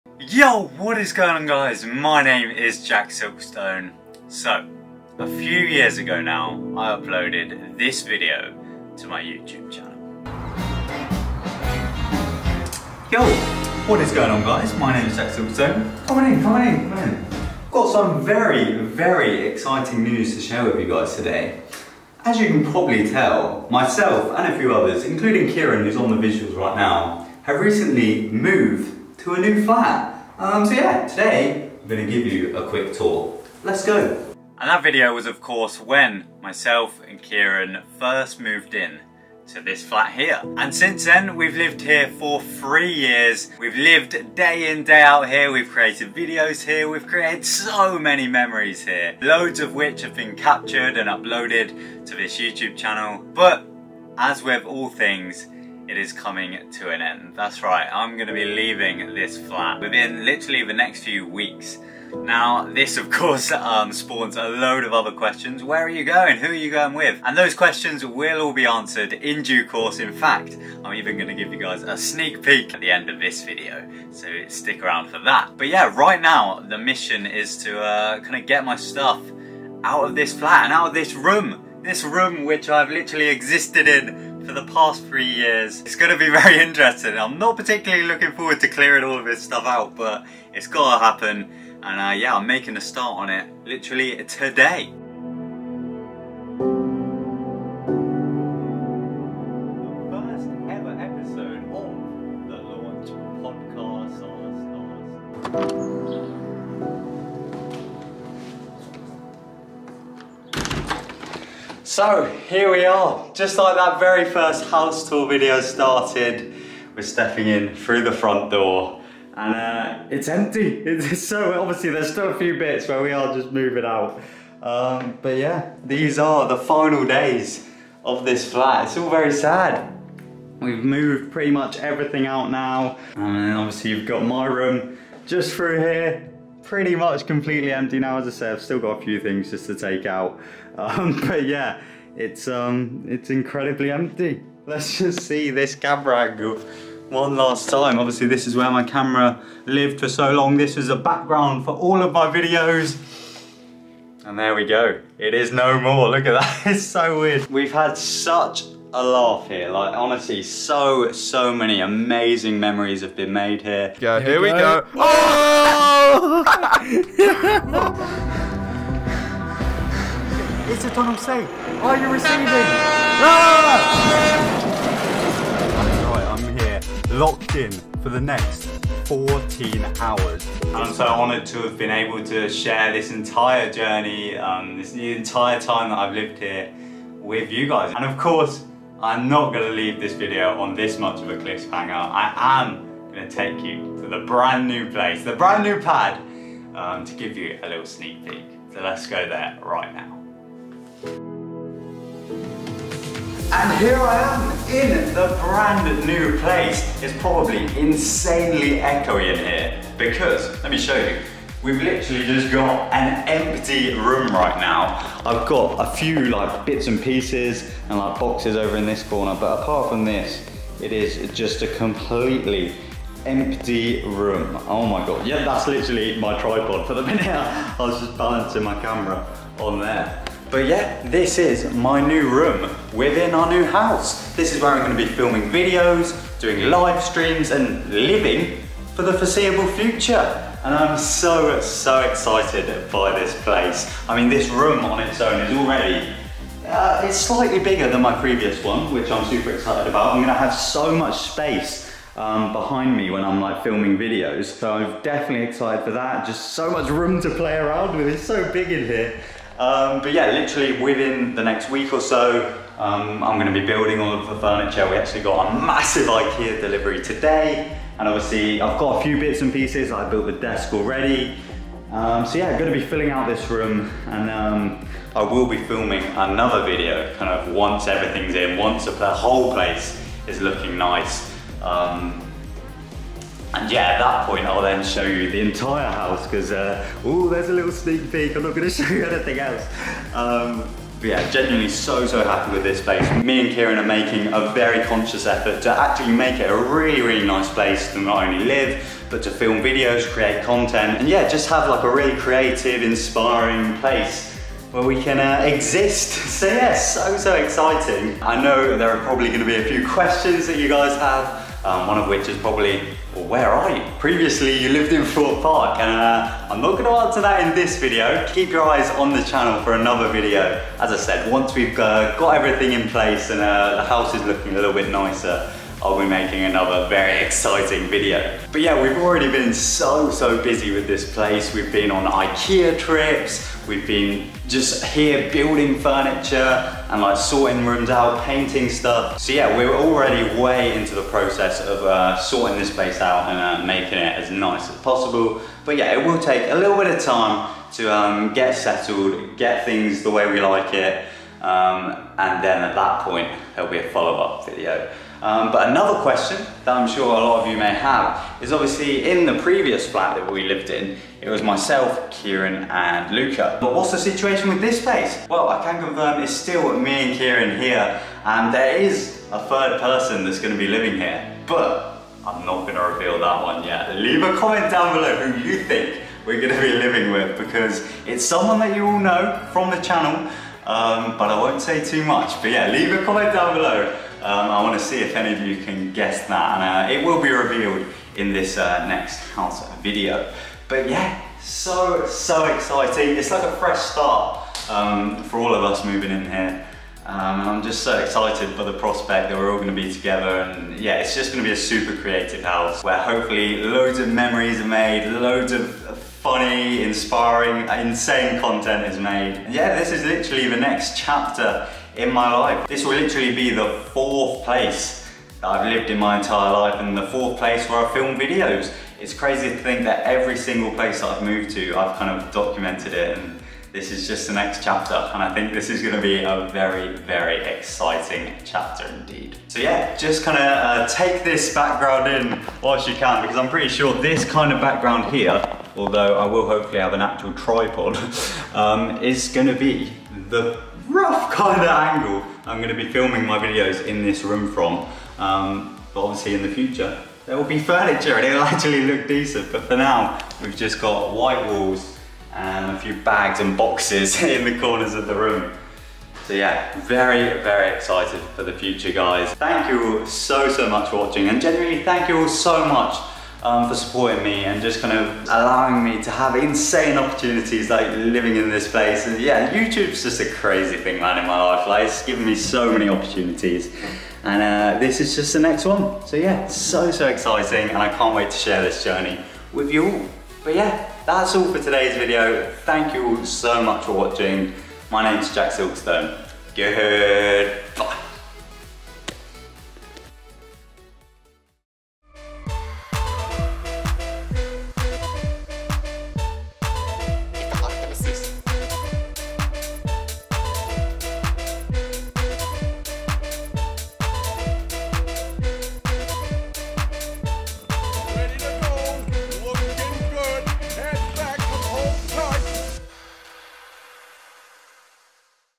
Moving Away from Thorpe Park...
This chapter of my life is coming to an end… Thorpe Park is my favourite theme park in the world but it’s time to MOVE on.